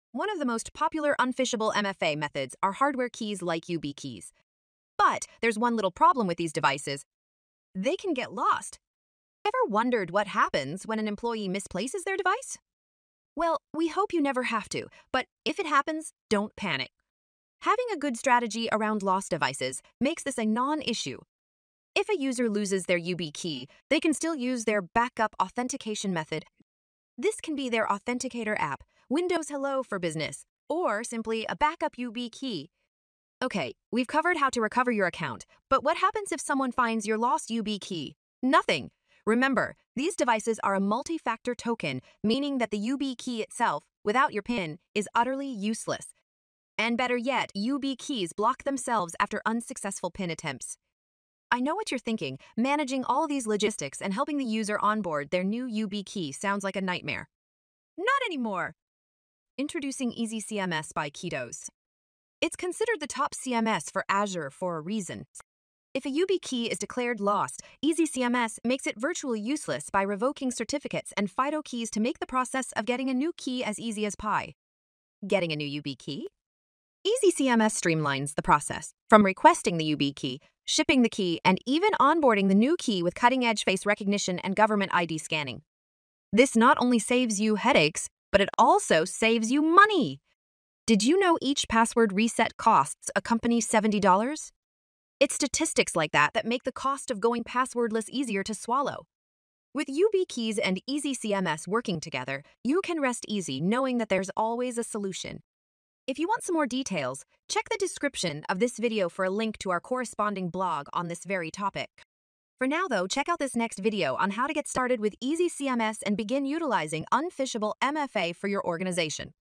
What Happens When You Lose Your YubiKey
What Happens when you lose your Yubikey? Can Attackers use a found Yubikey? No! Learn why in our new video and start protecting your identity with YubiKeys.